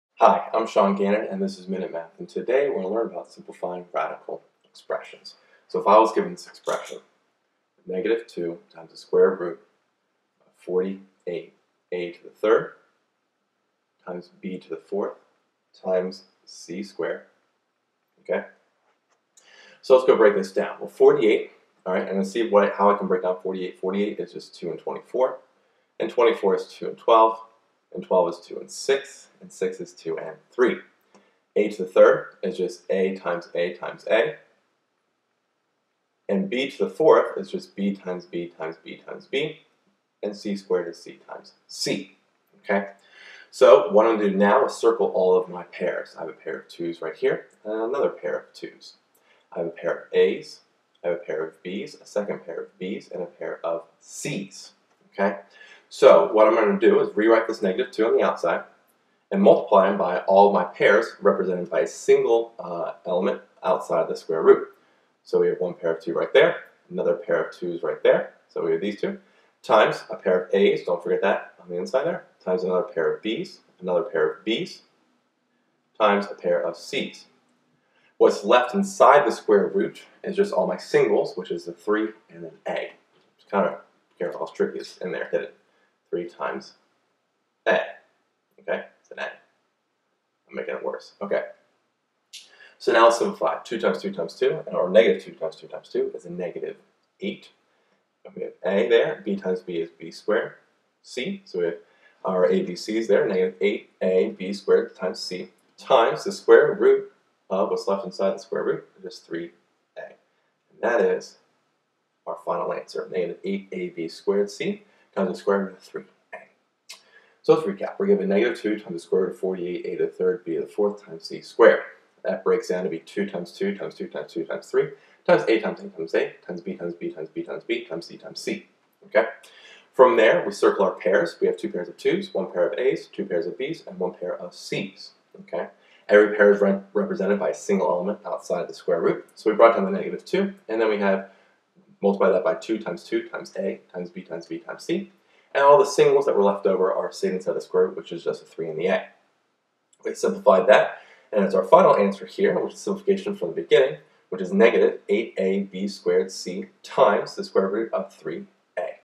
Simplify -2√(48a^3b^4c^2)
In this math video lesson on Simplifying Radical Expressions, I simplify -2 times the square root of 48a^3b^4c^2.